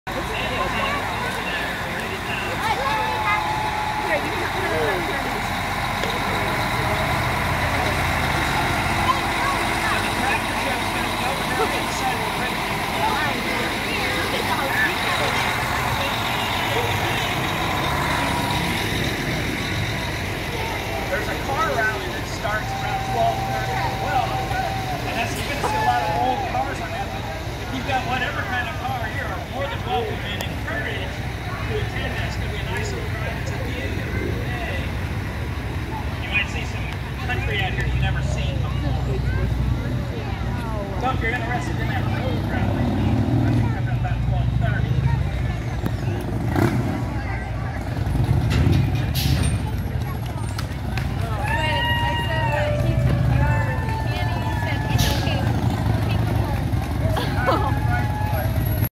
SCIO Oregon Festival Parade
2023 Lamb and wool festival parade.

with Ground Up Soil From Scio Oregon